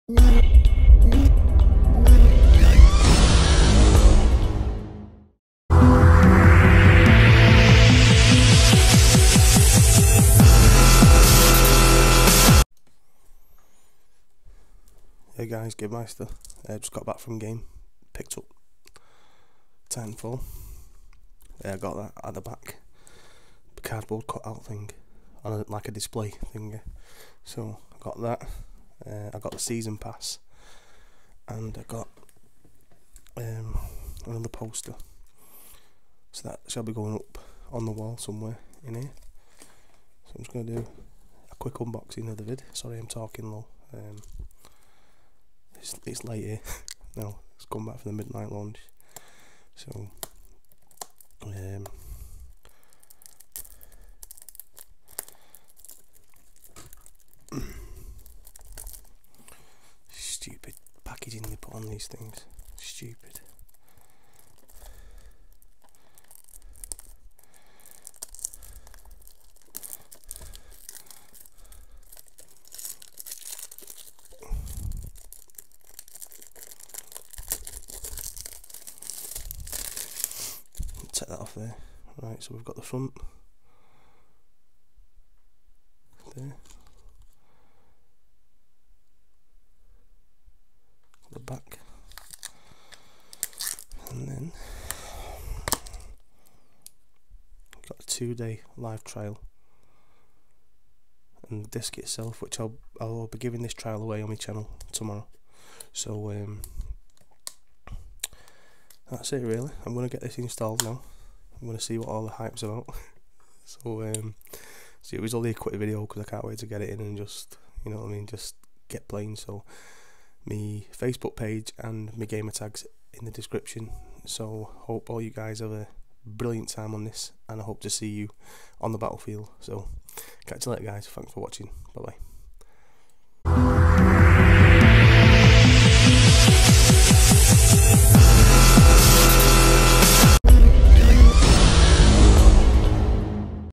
TitanFall Quick Unboxing | Xbox One
Very quick unboxing and a few Goodies lol
See you on the battlefield Gamers